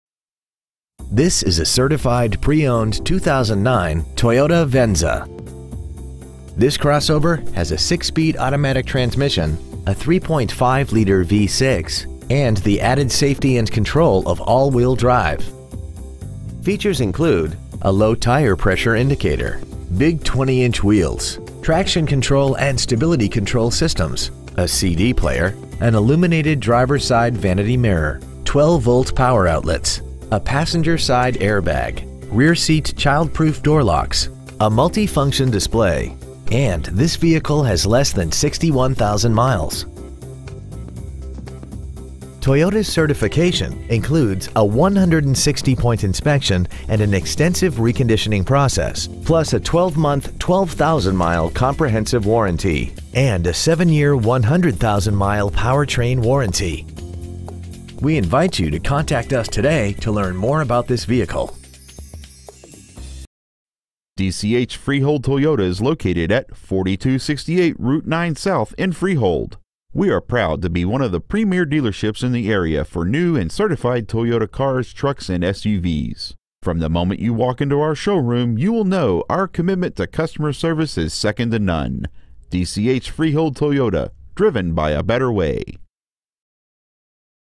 Certified 2009 Toyota Venza Freehold NJ 7728
We've been honored to serve the Freehold NJ area , we promise that your experience at our dealership will exceed your expectations ! Year : 2009 Make : Toyota Model : Venza Engine : Gas V6 3.5L/211 Trans . : Automatic Exterior : Magnetic Gray Miles : 60,218 Stock : FTR1798 DCH Freehold Toyota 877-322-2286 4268 Route 9 South Freehold , NJ 07728